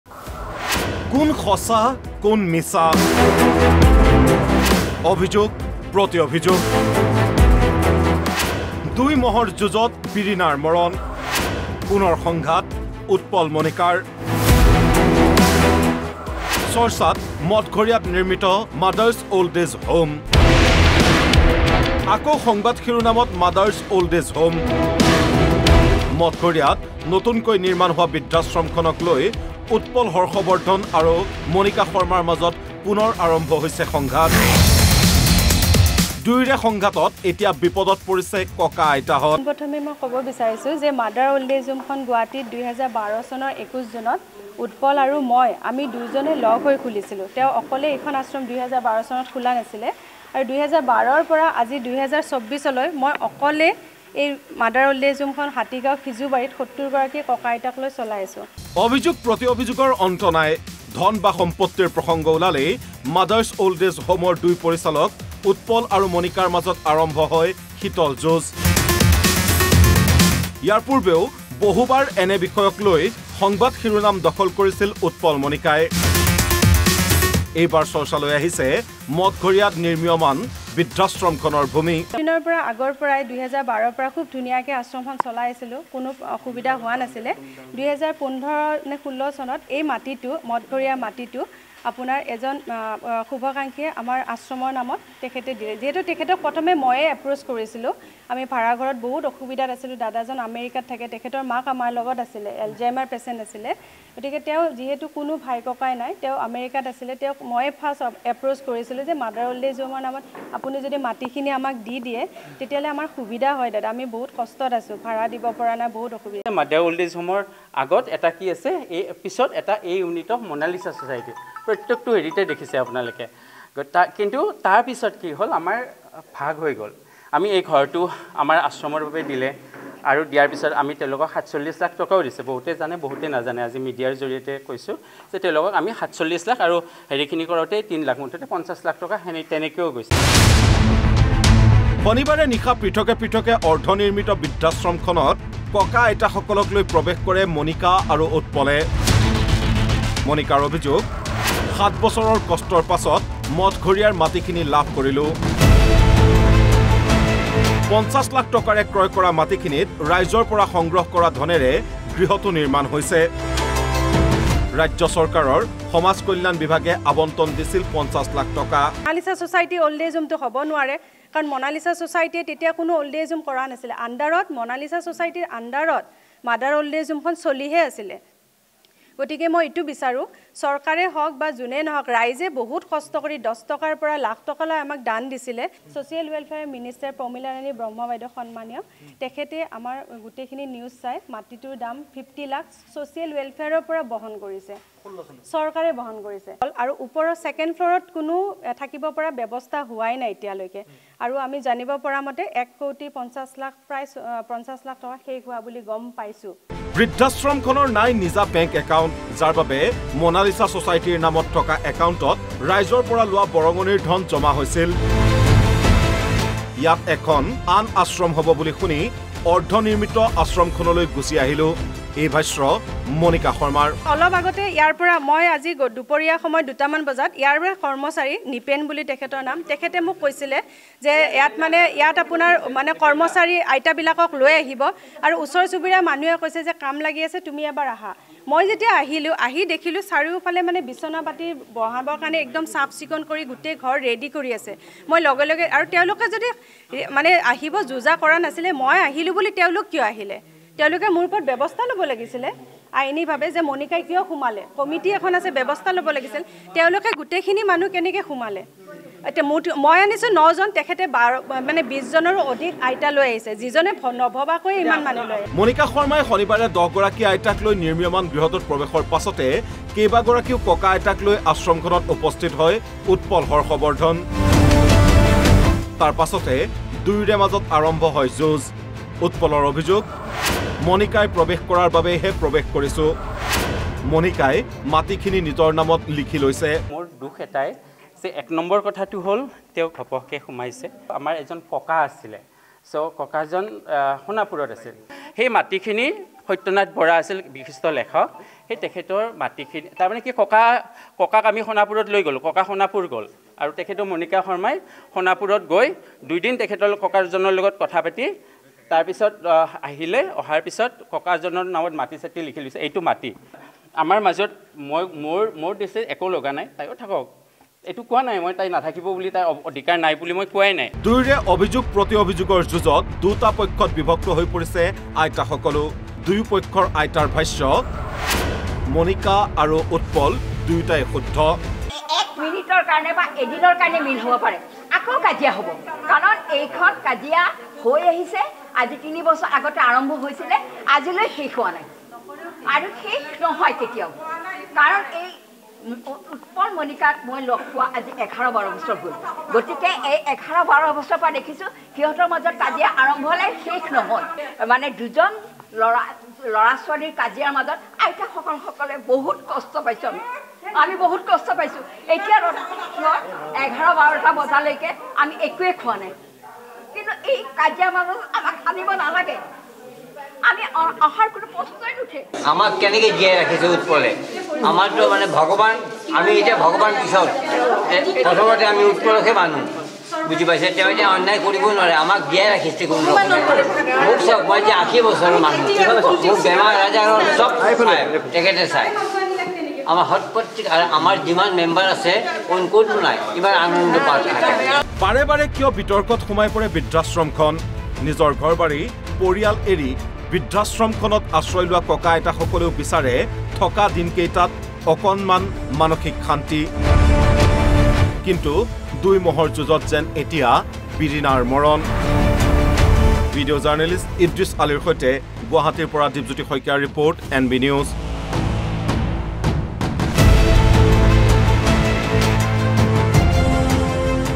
পুনৰ চৰ্চালৈ আহিছে 'মাডাৰ্ছ অল্ড এজ হোম'
পুনৰ চৰ্চালৈ আহিছে 'মাডাৰ্ছ অল্ড এজ হোম'৷ মঠঘৰীয়াত নৱনিৰ্মিত বৃদ্ধাশ্ৰমখনক লৈ পুনৰ সংঘাত আৰম্ভ হৈছে উৎপল হৰ্ষবৰ্ধন আৰু মণিকা শৰ্মাৰ মাজত৷ প্ৰকৃততে কি হৈছে মঠঘৰীয়াত নিৰ্মীয়মান বৃদ্ধাশ্ৰমখনত?